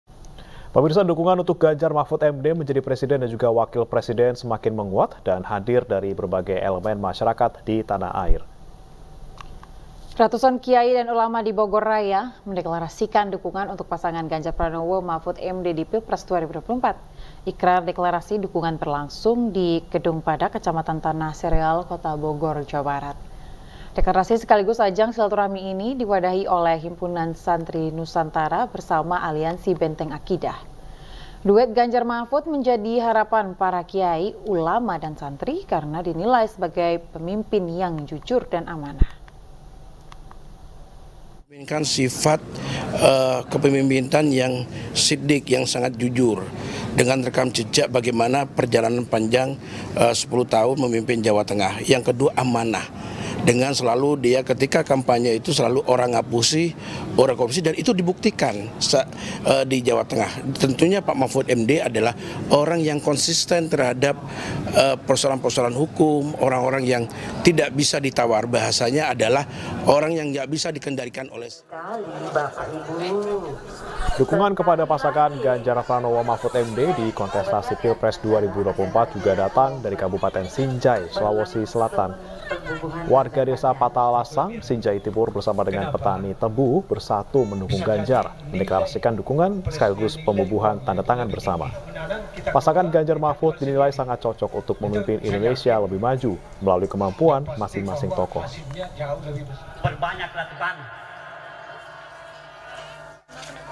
Ratusan Kiai dan Ulama Se-Bogor Raya Deklarasikan Dukungan untuk Ganjar-Mahfud di Pilpres 2024
Dukungan untuk pasangan Ganjar Pranowo dan Mahfud MD menjadi presiden dan wakil presiden semakin menguat dan hadir dari berbagai elemen masyarakat di Tanah Air. Dukungan resmi untuk Ganjar-Mahfud hadir dari ratusan kiai dan ulama se-Bogor Raya.

Mereka hadir dari berbagai daerah di Bogor. Ikrar deklarasi dukungan mereka kukuhkan di Bogor Valley Hotel di Kedungbadak, Kecamatan Tanah Sereal, Kota Bogor, Jawa Barat, Sabtu (4/11/2023). Deklarasi ini diwadahi oleh Aliansi Benteng Akidah (ABA) bersama Himpunan Santri Nusantara (Hisnu).